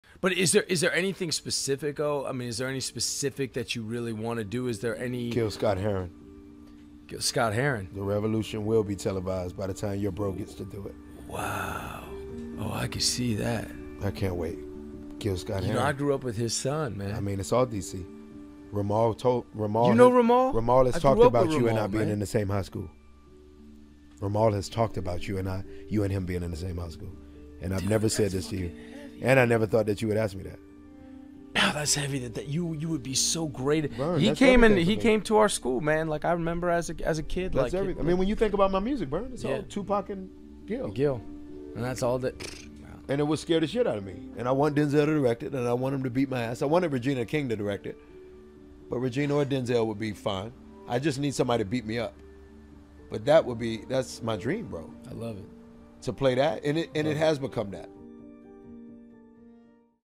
Omari Hardwick’s dream role
Omari Hardwick explains how the producers at Starz and team of producers such as 50 Cent got behind him for the role, and what it was like as the show led to multiple spinoffs and became the network's tentpole series.